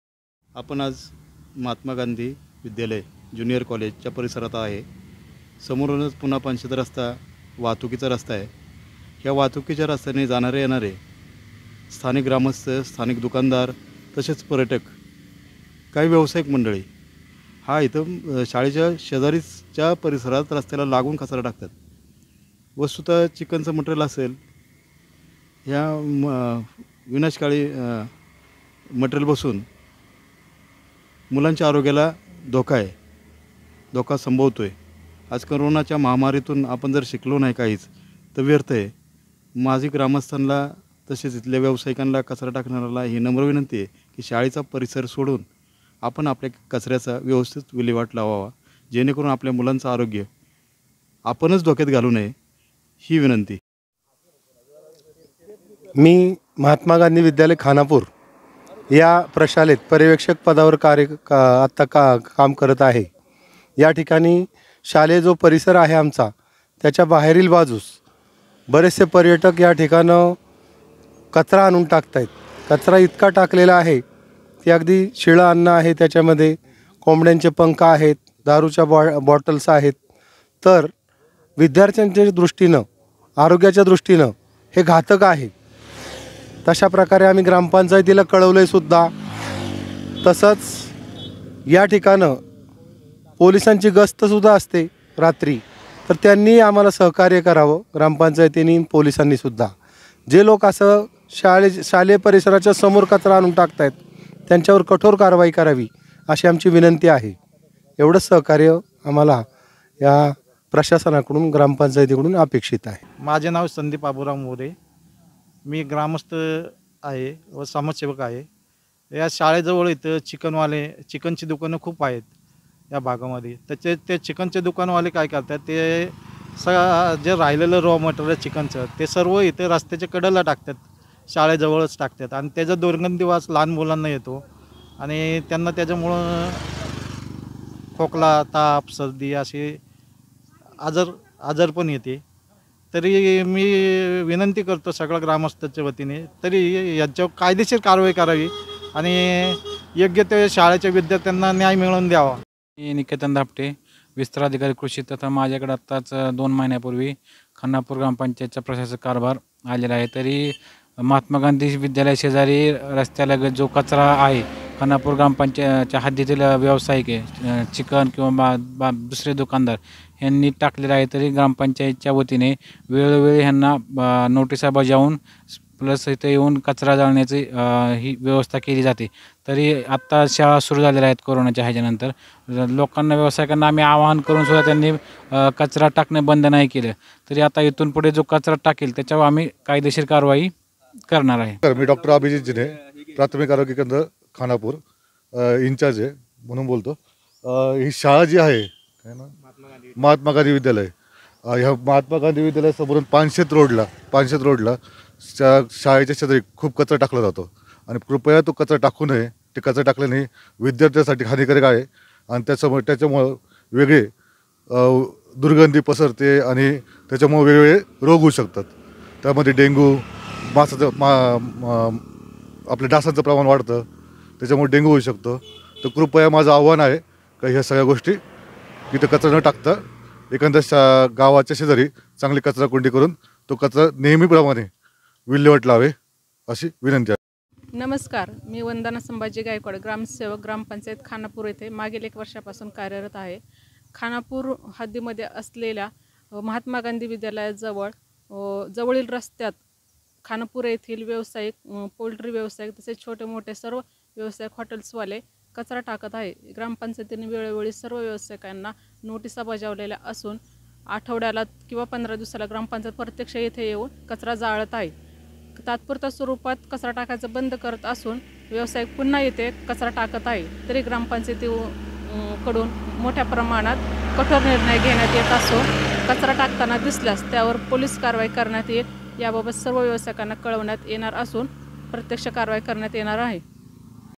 खानापूर | विद्यालयासमोर टाकण्यात येणाऱ्या कचऱ्यामुळे विद्यार्थ्यांचे आरोग्य धोक्यात
खानापूर | विद्यालयासमोर टाकण्यात येणाऱ्या कचऱ्यामुळे विद्यार्थ्यांचे आरोग्य धोक्यात, प्रशासनाचे दुर्लक्ष सविस्तर बातमी